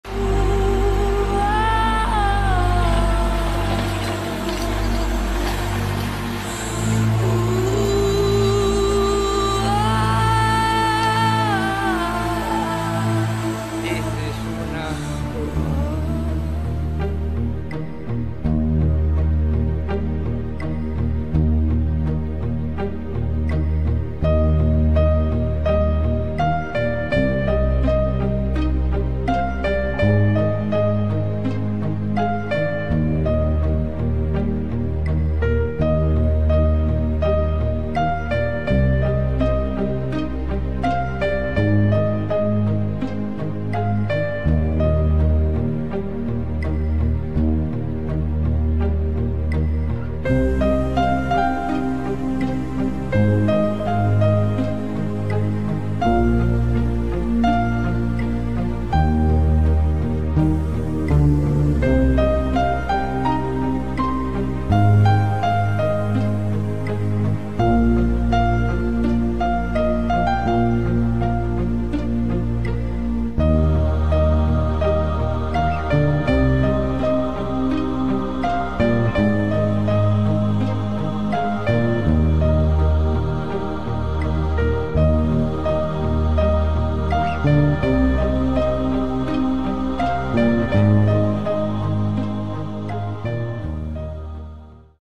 Plastik im Meer Umweltverschmutzung Klimaschutz
Nachhaltigkeit geht uns alle an. Dieses Video zeigt die langsame Zerstörung unseren Planeten. Aktiviert die Glocke um nichts mehr zu verpassen. Last bitte ein Like da. Und über ein Abo würde ich mich sehr freuen danke.